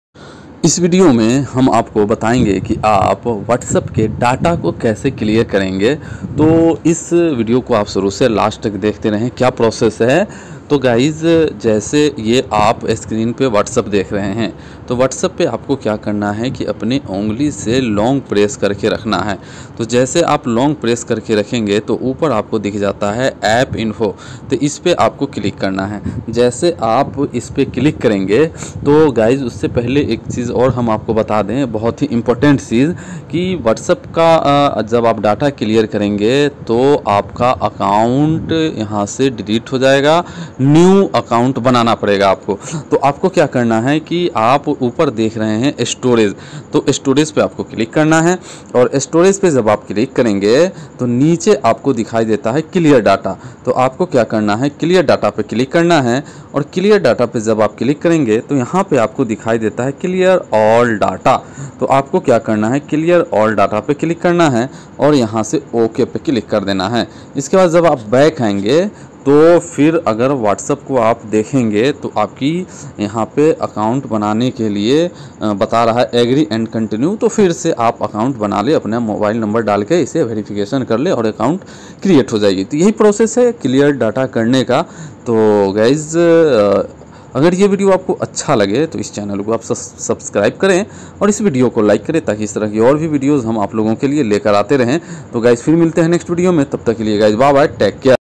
WhatsApp Ka Data Clear kaise Kare | How Data Clear in WhatsApp
Is Video me Mai Aap ko Bataya hu ki Aap WhatsApp ka Data Clear kaise Kare